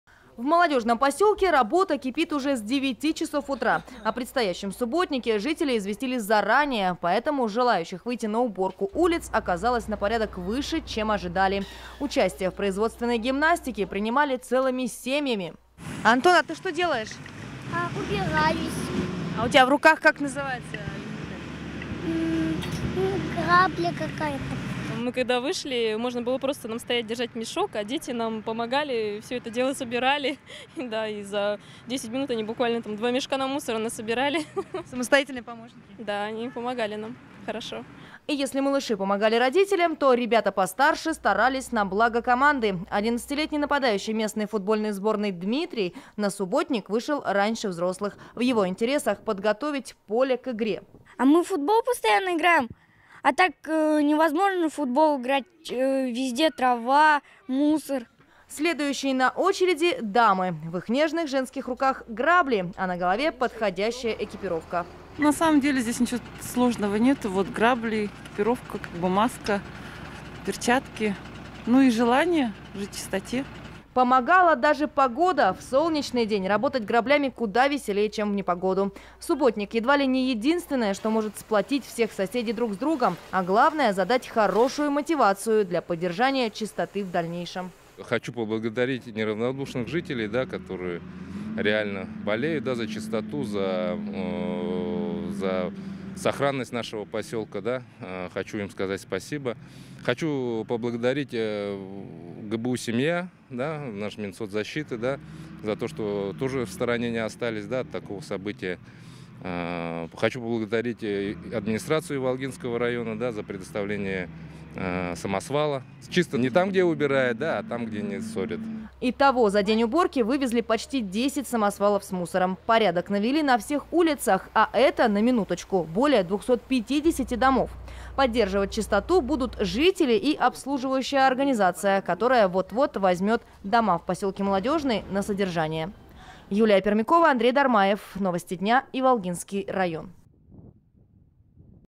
Субботник
В микрорайоне Молодёжный очистили от мусора улицы и дворы